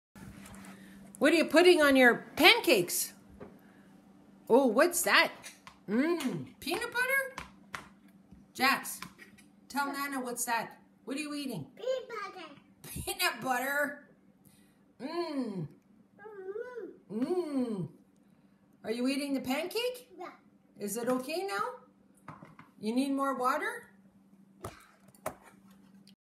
Jax eating pancakes at nanas
saying the word fork